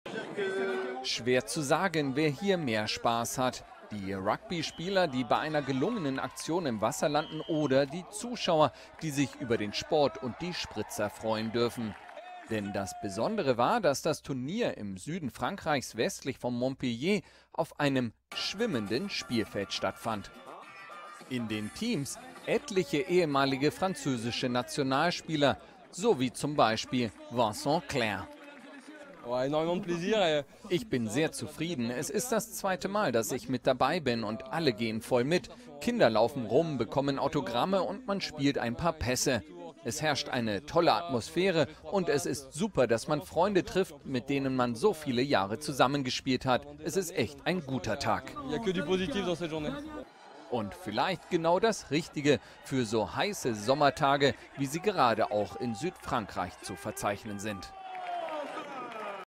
Rugby mal anders: Punkt, Sprung, Spritzer
Ein außergewöhnliches Turnier mit etlichen französischen Ex-Nationalspielern fand auf einem schwimmenden Spielfeld in Südfrankreich statt.